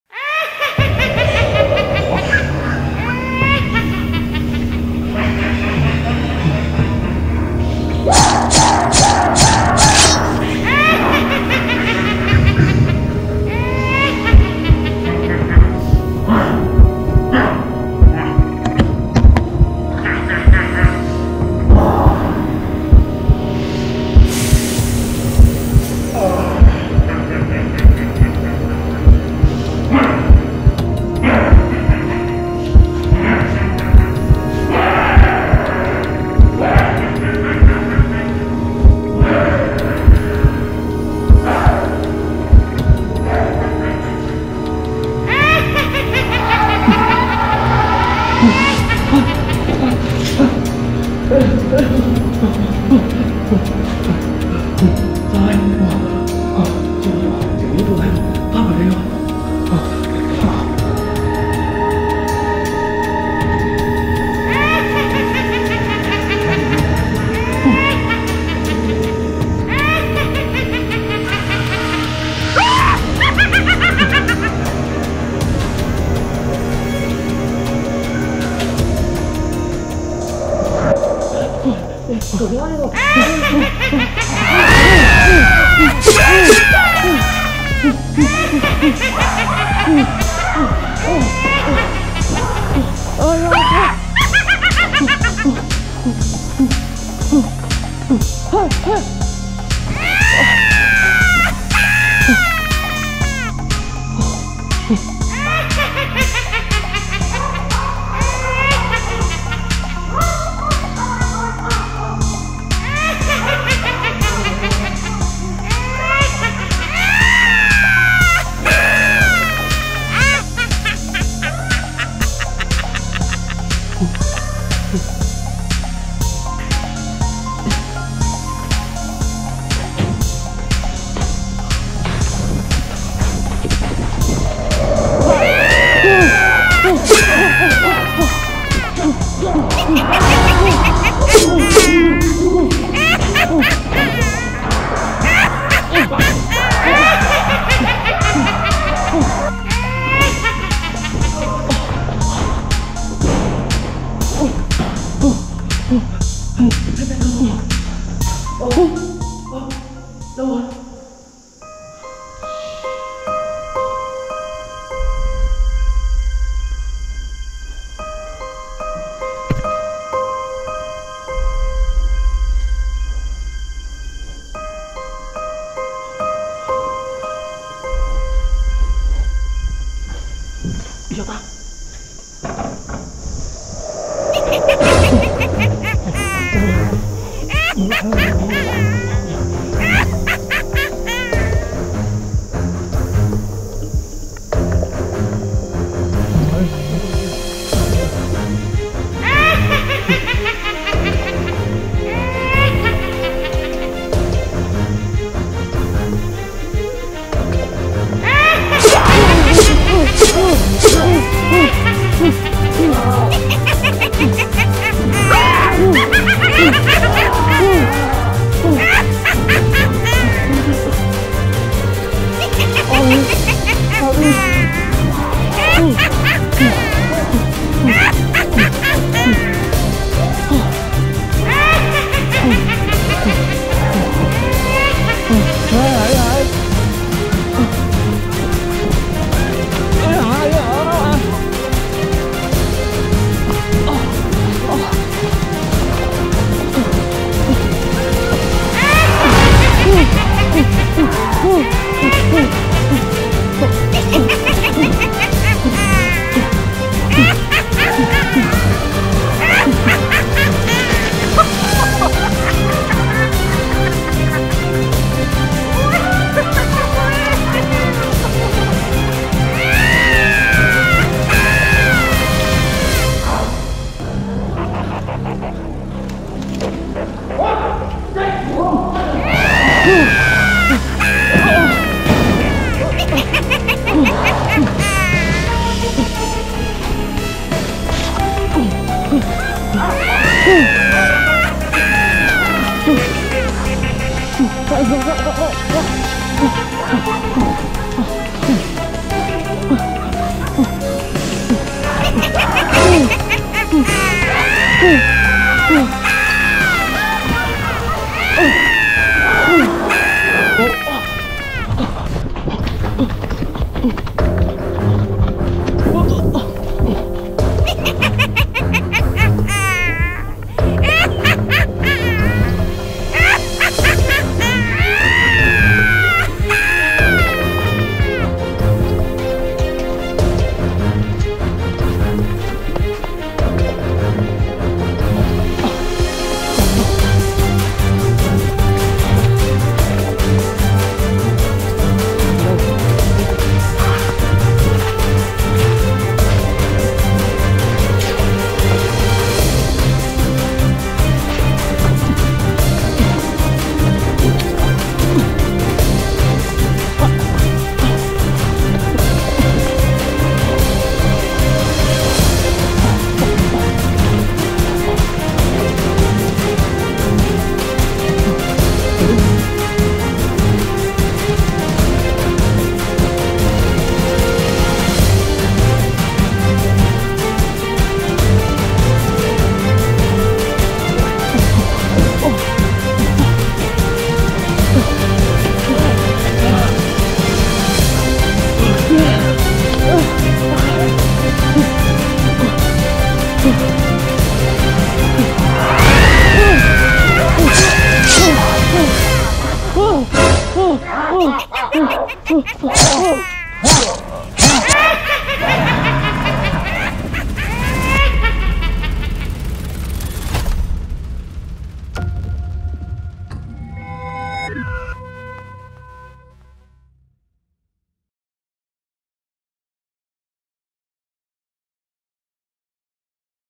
SERBIAN DANCING LADY REAL LIFE ESCAPE 3.0 | Horror Parkour Pov Short | Bubbles 3Run
The Serbian dancing lady is described as a middle-aged woman. A woman can be seen dancing a strange dance at night anywhere in Serbia and if anyone sees her, or wants to get close, she will take out the knife that is already on her person. Chase. Somehow, wherever he/she runs, she appears with her speed and devilishness, no one can escape her blade.

Escaping Crazy Serbian Dancing Lady Horror

Parkour Pov

The action scenes in the video were done by experts with many years of experience, please do not follow them. Thank you!!!